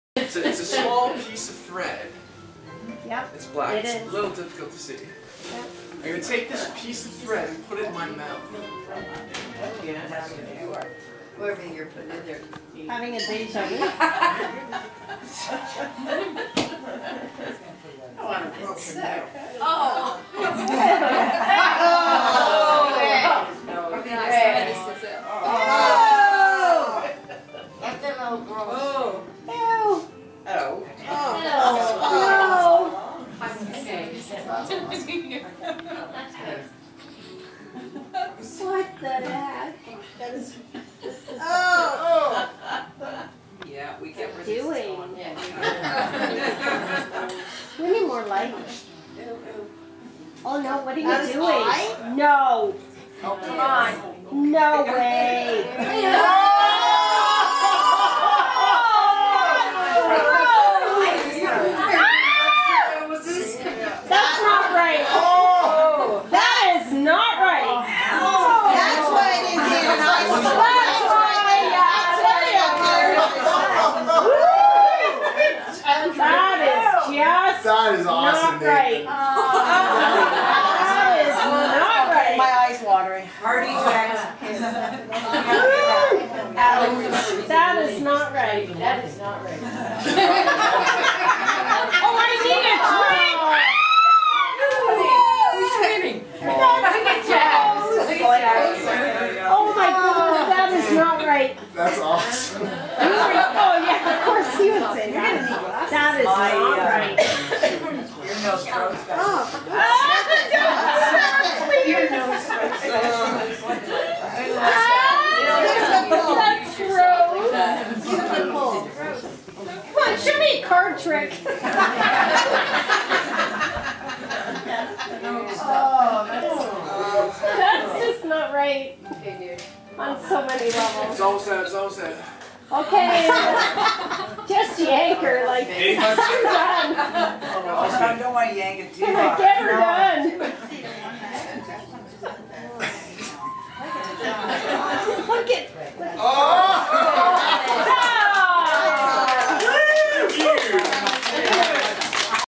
Thread from Eye! - Ashtar Avatar
Ashtar Avatar puts a thread in his mouth and pulls it out of his eye!